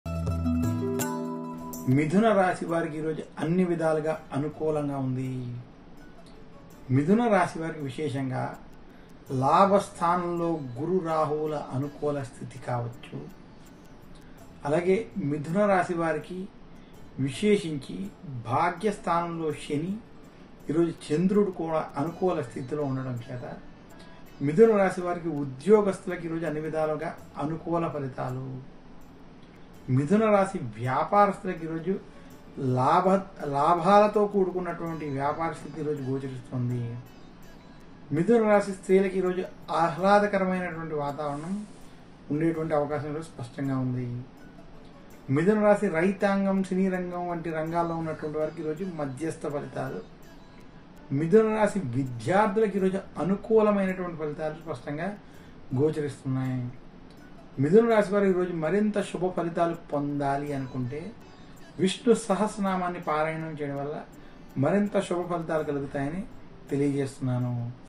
Daily Horoscope : మిధున రాశి వారికి ఇవాళ ఎలా ఉంది..? | 06-09-2023 - TV9 Digital
Horoscope Today, September 6 , 2023 : Daily Astrological Predictions